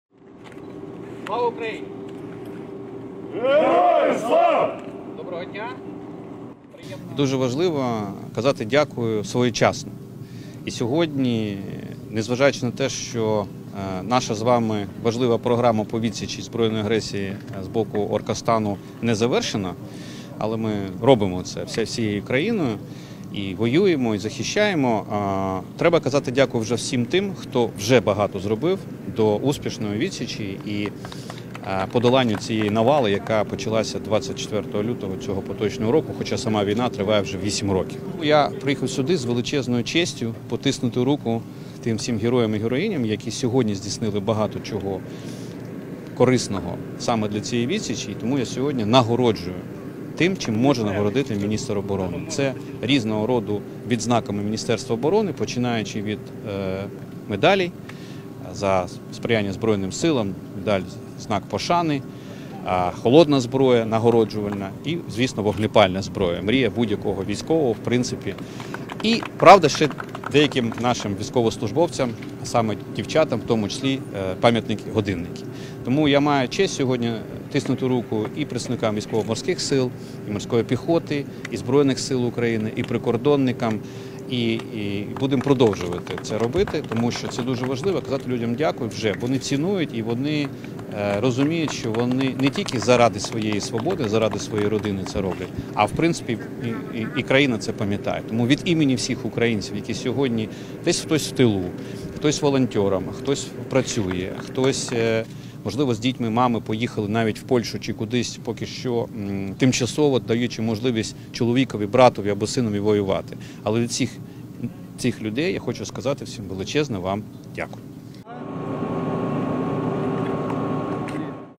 В Одесі Міністр оборони України нагородив захисників України вогнепальною та холодною зброєю
Перебуваючи в Одесі Міністр оборони України Олексій Резніков нагородив кращих військовослужбовців Військово-Морських Сил Збройних Сил України, які зробили значні внески у справі захисту країни.

- Не зважаючи на те що наша з вами спільна справа по відсічі ворога ще триває і ми всією країною воюємо та захищаємо, треба казати дякую всім тим, хто вже багато зробив для успішного захисту і подоланню російської навали, яка розпочалась ще у 2014 році і триває вже 8 років з моменту окупації АР Криму та продовжується відкритим вторгненням, - наголосив Олексій Резніков. - Тому я приїхав сюди з величезною честю потиснути руку тим героям і героїням, які сьогодні внесли значний вклад для цієї відсічі, - дода він.

Відео: телеграм-канал ВМС ЗСУ